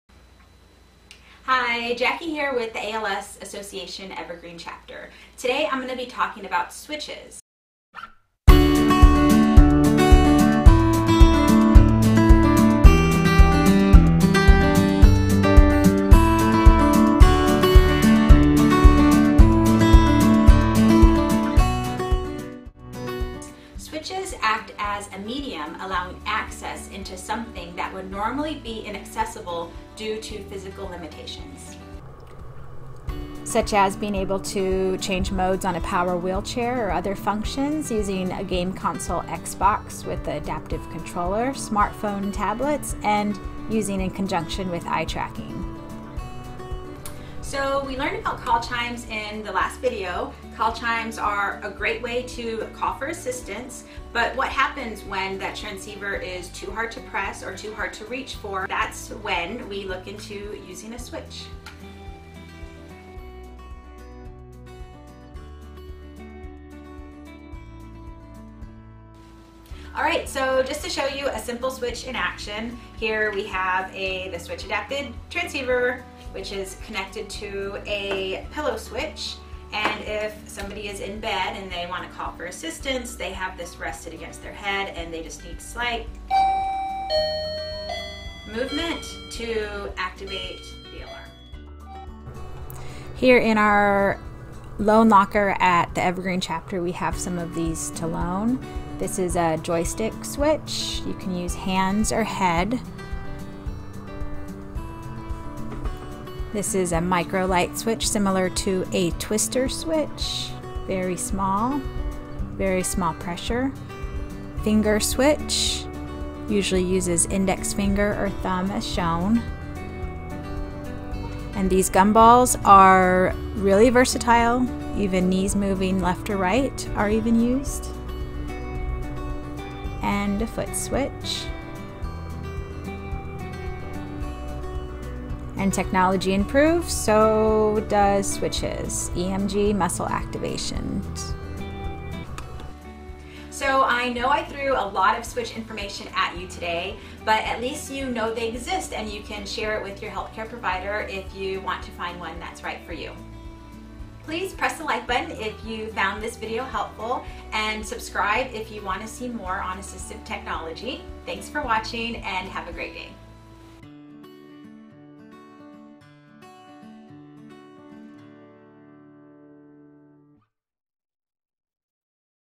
Assistive Technology Video Series: Switches
Jackie gives an introduction to switches that may assist
in someone accessing the environment if physical limitations occur.

List of switches demo'd in video, not for endorsement, just to be used as a guide:
Gumball Switch, Microlight Switch, Twister Switch, Joystick Switch, Finger Switch, Foot Switch, EMG Switches (Tinkertron & Neuroode by Control Bionics)

Ways to obtain loaner switches and AT Assessments if you have ALS in WA, ID, AK, and MT:

Power Wheelchairs: see your ATP Vendor

If you live outside our service region and are interested in obtaining a switch, please contact your local ALS Association Chapter, you can find a complete directory